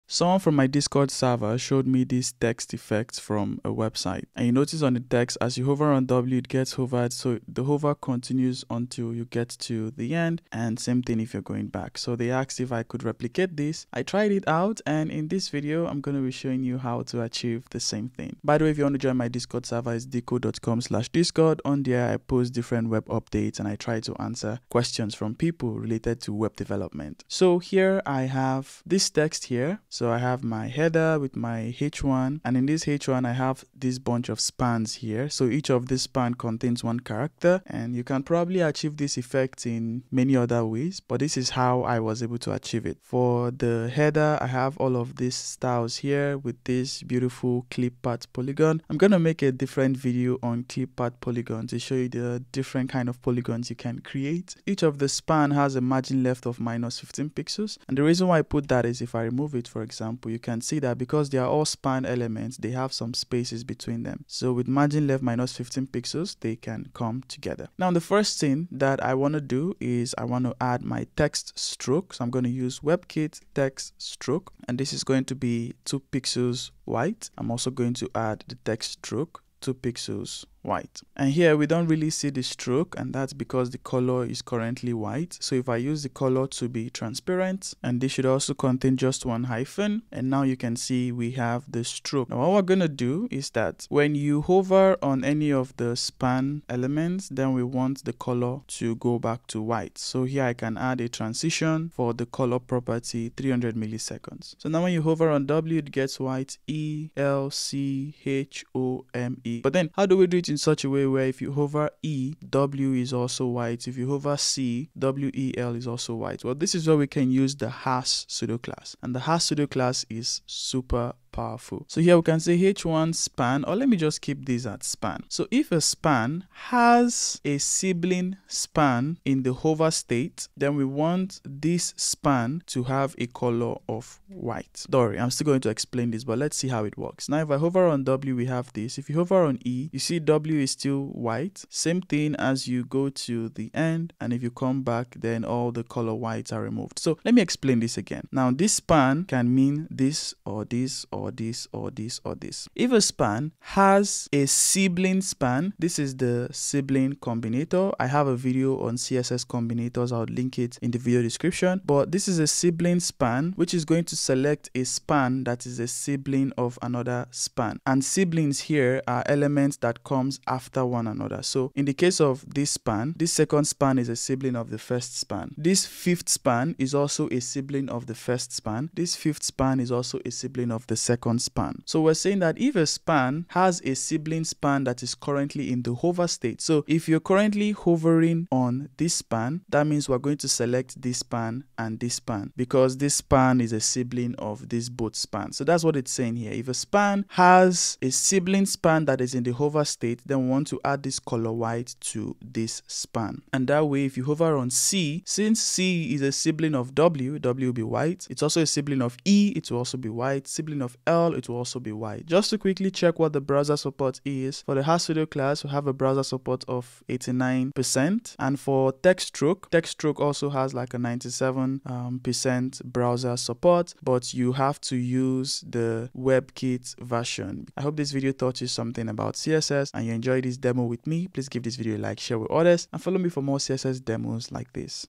Amazing Hover Effect with CSS :has Pseudo Class
The :has pseudo class in CSS can be used for a lot of interesting styles with just CSS. In this video, I show you how to use this pseudo class to achieve a nice hover effect on a text.